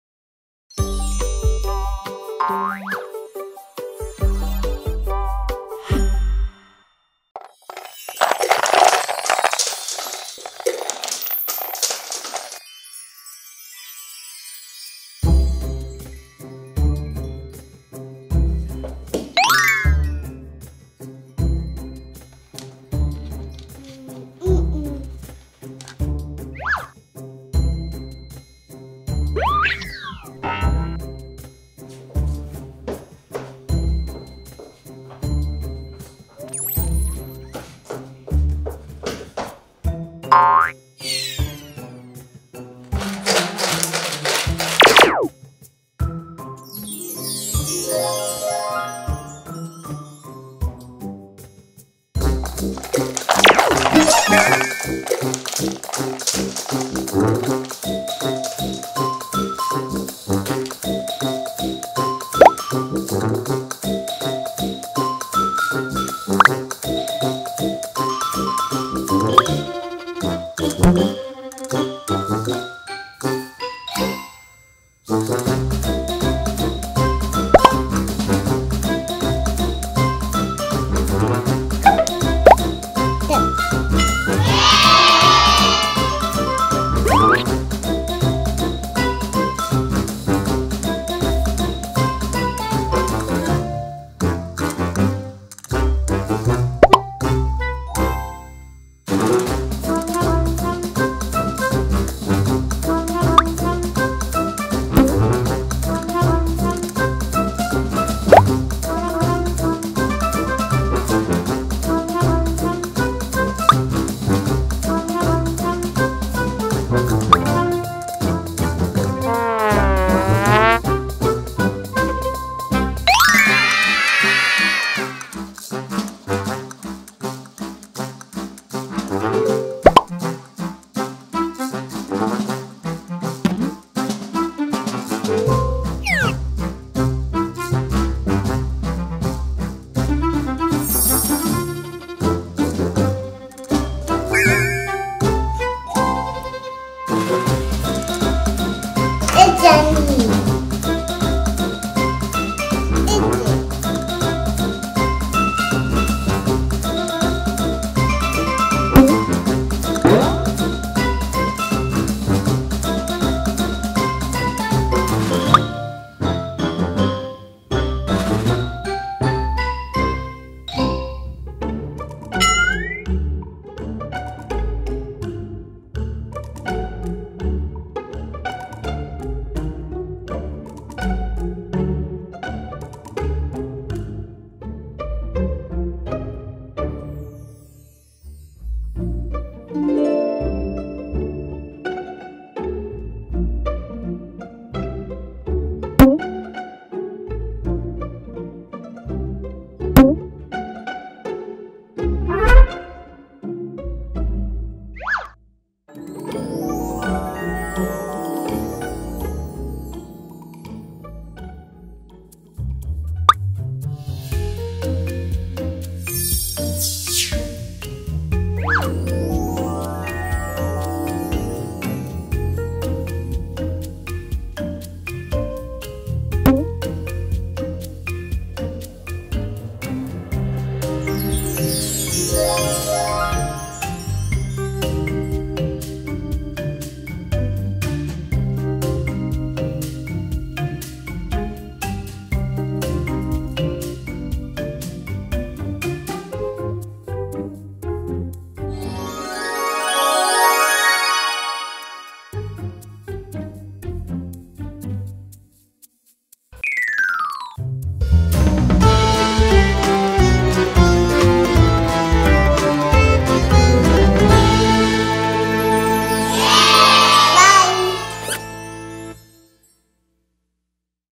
Lucy pretends to play with Bracelets and Necklaces
Lucy pretends to play with a new Game to assemble platics colored Bracelets and Necklaces
by Lucy and Brothers